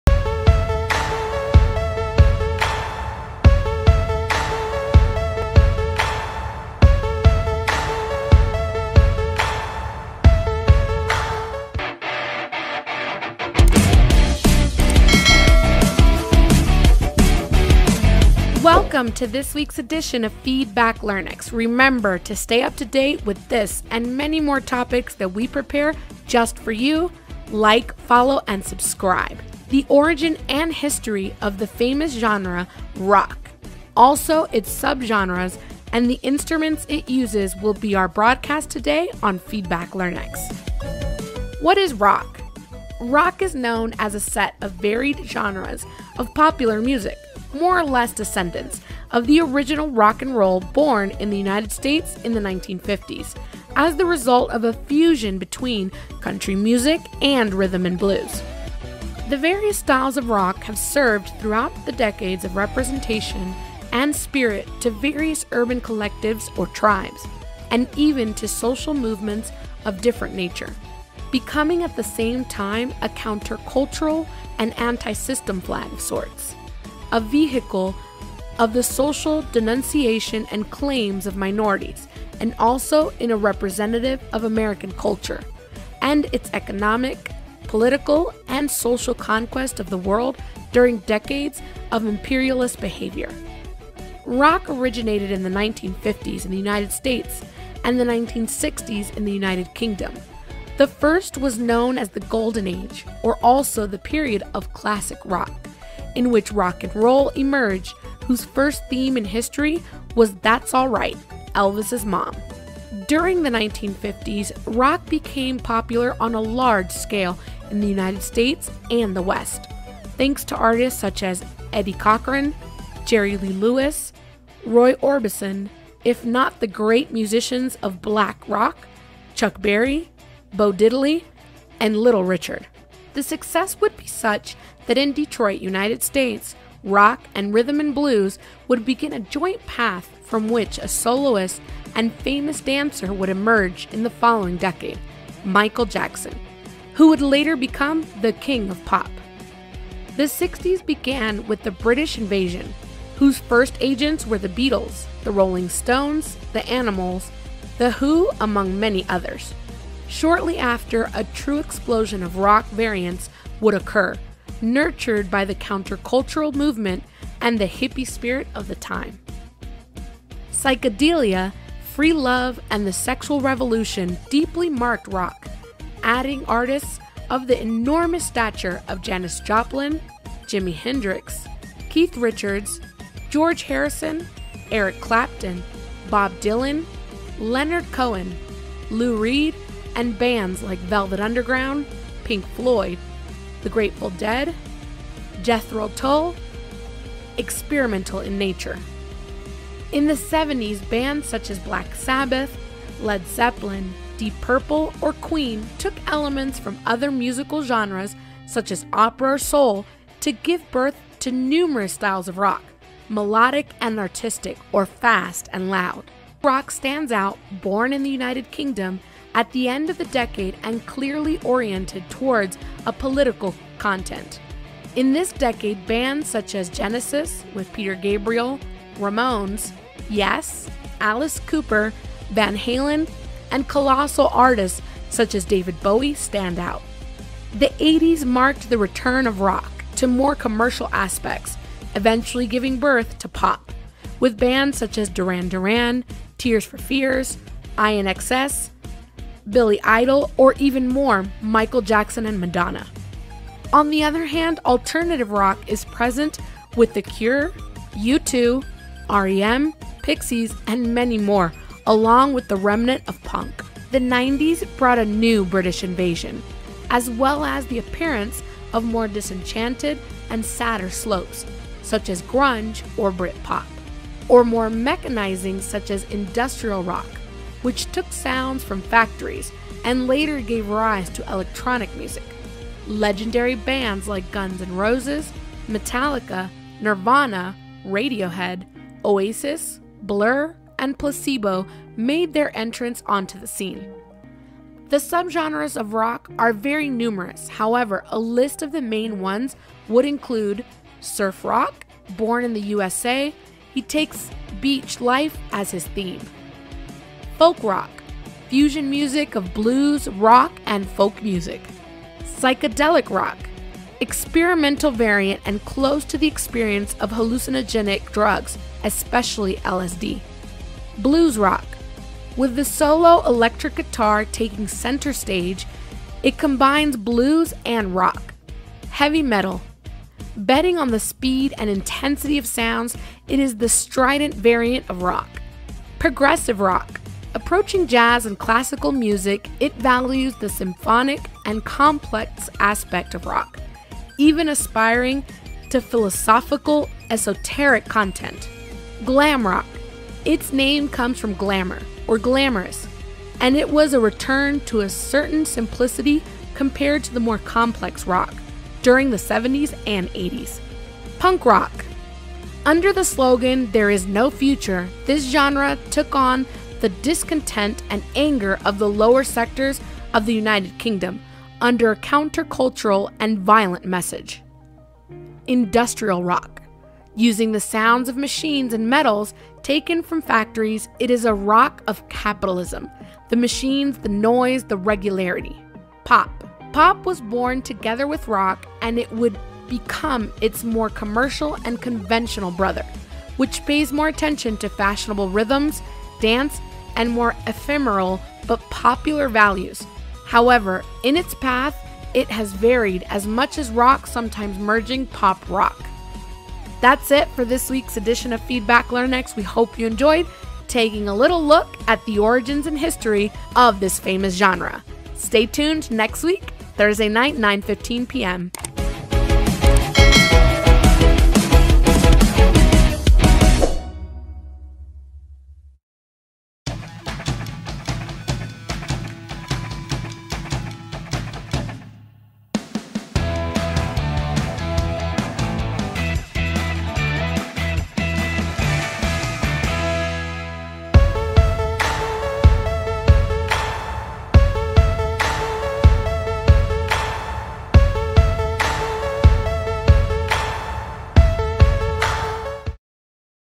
ORIGINS AND HISTORY OF ROCK MUSIC
El Rock es un género musical que se caracteriza por tener letras con gran significado, derivadas de otros estilos musicales como el Jazz, Blues, Country y Folk, desde su nacimiento hasta la actualidad, el Rock ha sido parte de diversos cambios y mezclas que lo han dotado. con numerosos matices. Haz clic aquí en Feedback Learnex y maravíllate con este gran género musical.